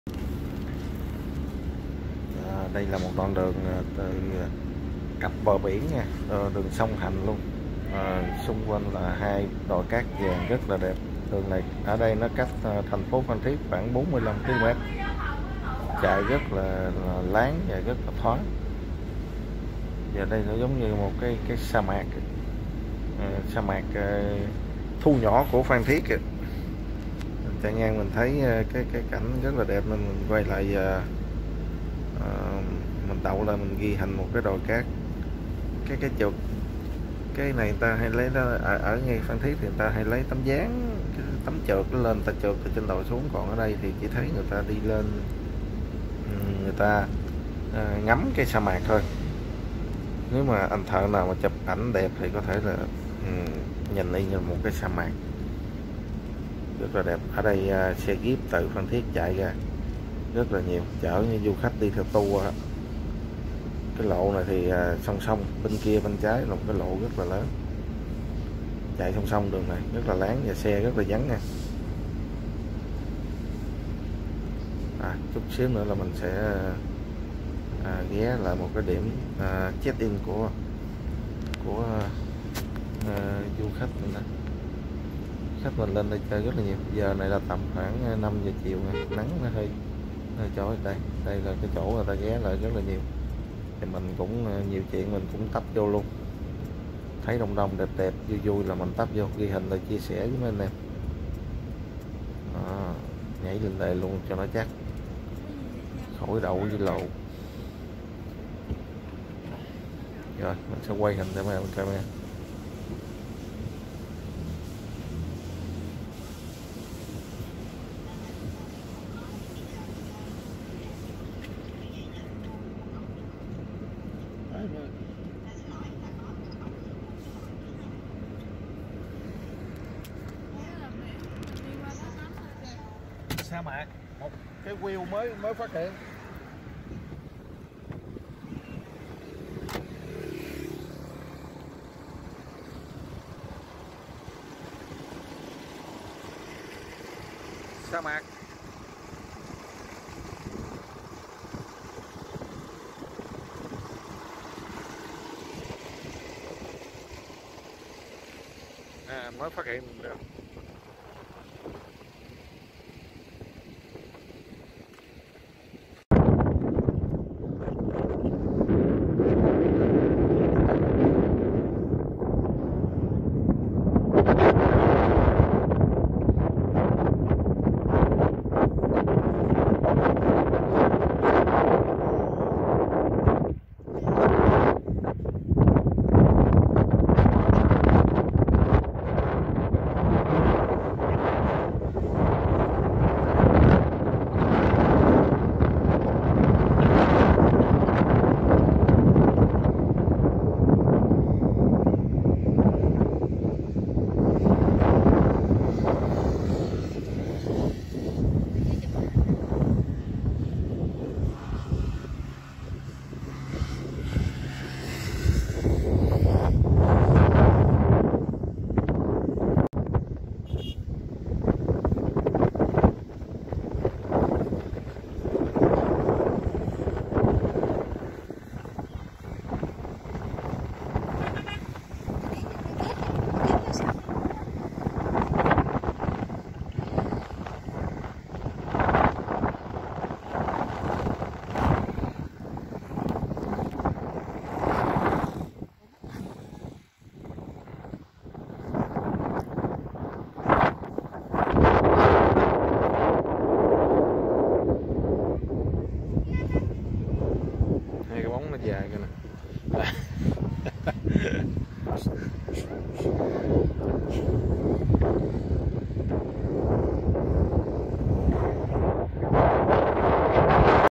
# 30 : Sa Mạc Phan Thiết
Đoạn đường cặp bờ biển rất dài và đẹp, cách TP Phan Thiết khoảng 45km về hướng Nha Trang, có 1 đoạn rất nhiều khách du lịch ghé chụp hình . một Sa Mạc thu nhỏ của Phan Thiết